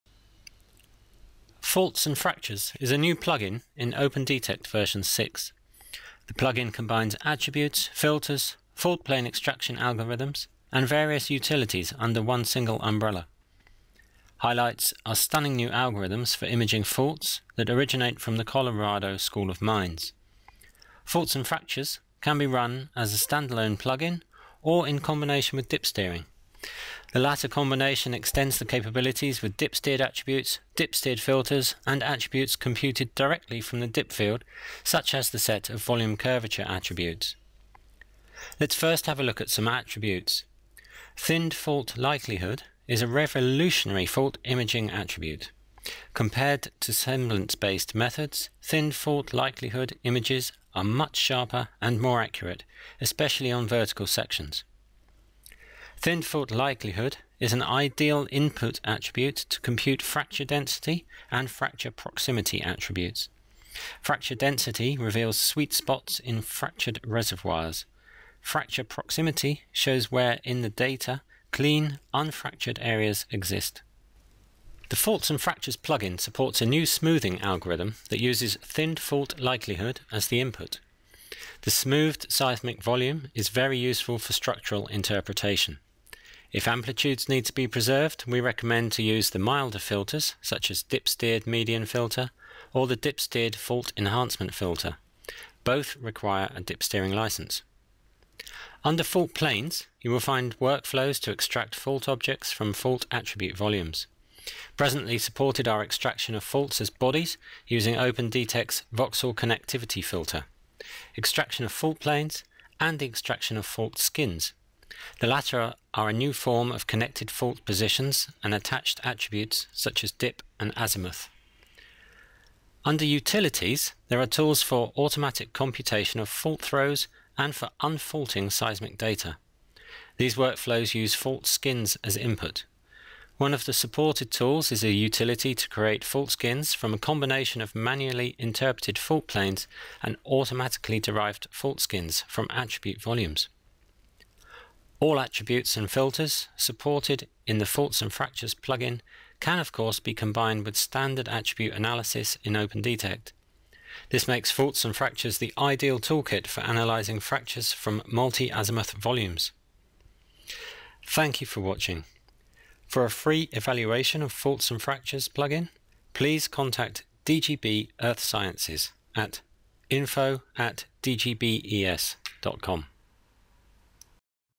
New in OpendTect 6: Faults & Fractures Plugin
Video about the new Faults & Fractures Plugin in OpendTect 6. It shows the new plugin, stunning new algorithms and more.